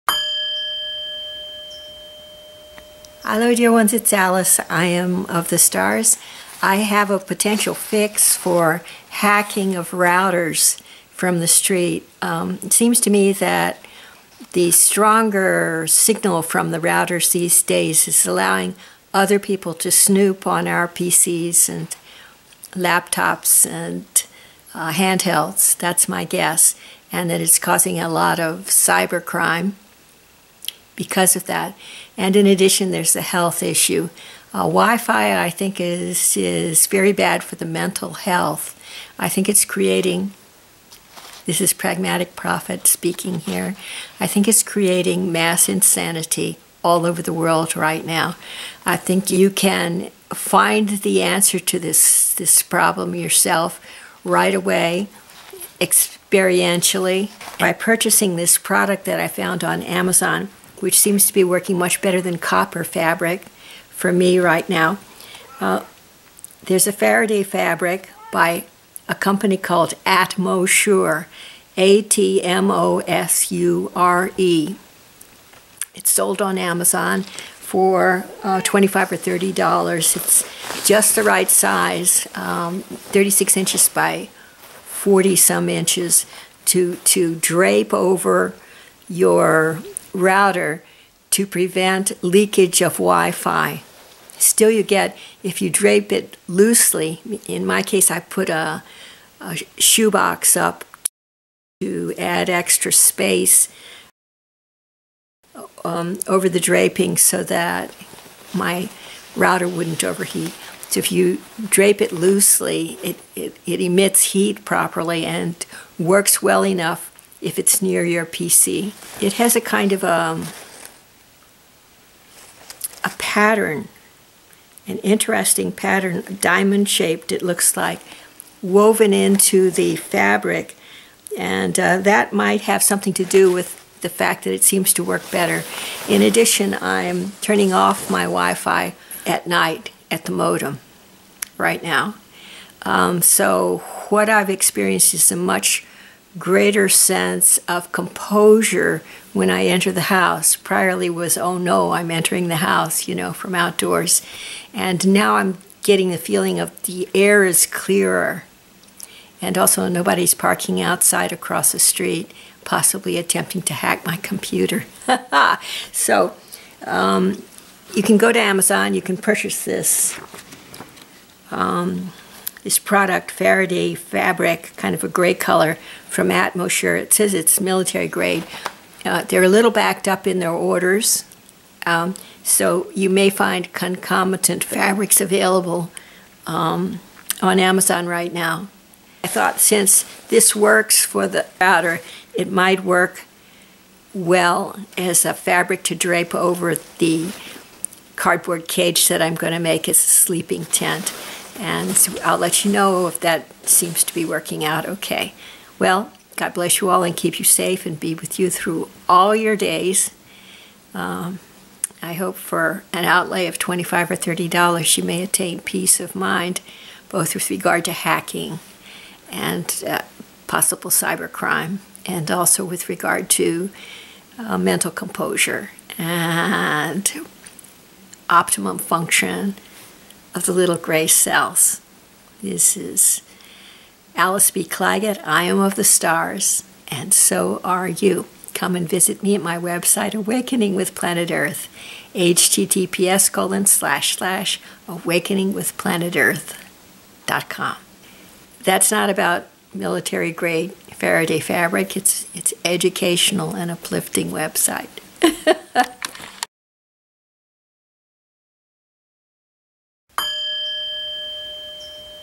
Faraday Fix for Hack Attacks and to Quell Wi-Fi Cognitive Impairment
Filmed and published on 28 February 2024

Erratum: The correct title contains the word "Quell" and not the word "Prevent."

WI-FI CONTAINMENT INFORMATION
(I get no money for this - I just like the product.)

ATMOSURE Faraday Fabric (36" x 44")
at Amazon.com
This works great for me if I drape it over my router.
I tried using it on my head and torso last night, and I feel that, for me personally, it doesn't feel quite right. Copper fabric from Amazon.com is a little better; but again, I personally don't feel comfortable with that for more than half an hour, once in a great while.

VIDEO CREDITS

Filmed and produced on 28 February 2024